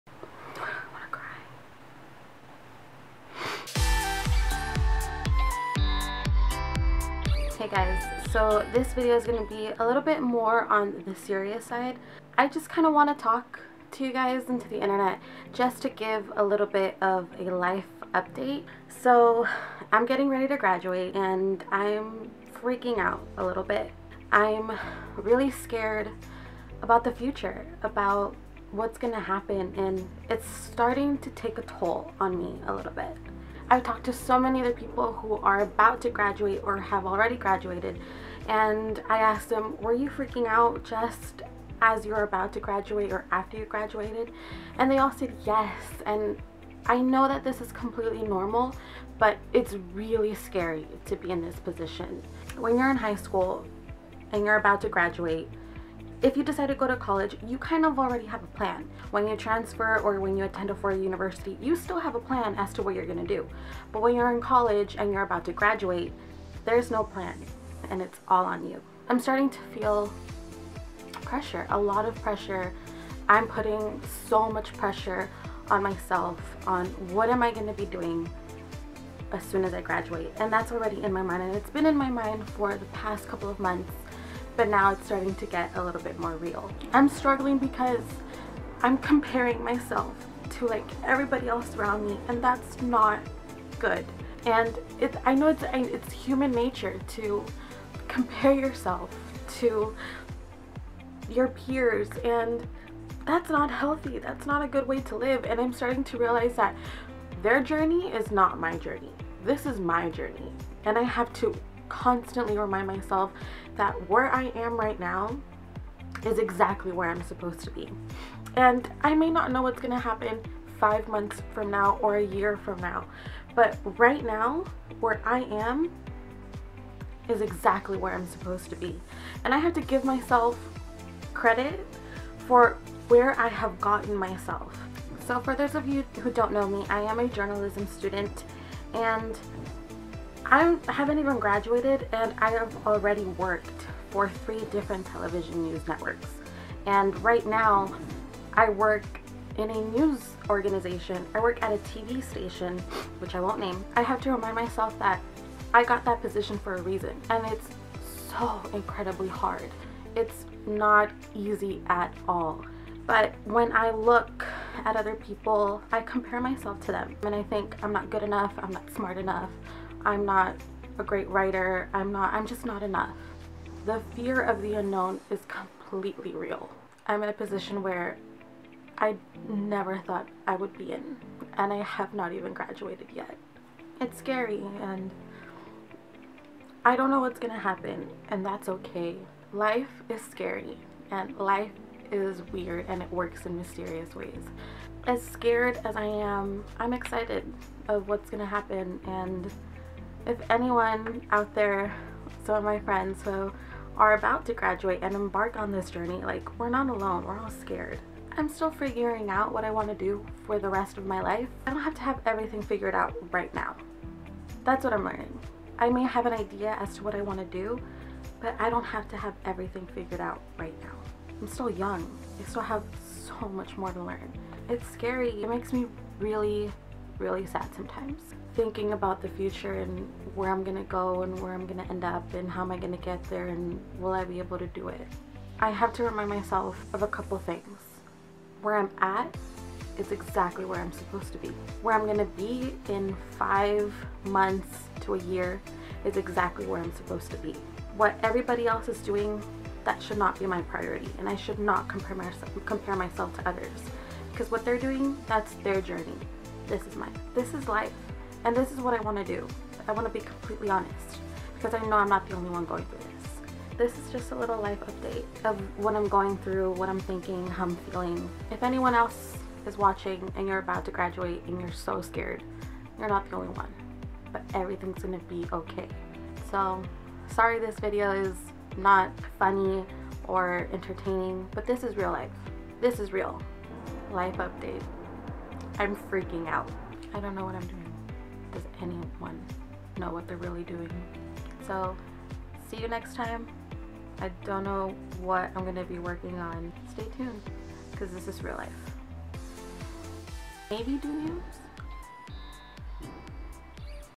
VLOG: I Don’t Know What’s Going To Happen | Elizabeth Rodriguez
Vlog #3

I Don’t Know What’s Going To Happen

Like most college seniors I am nervous about what's going to happen after I graduate. Whatch me cry here.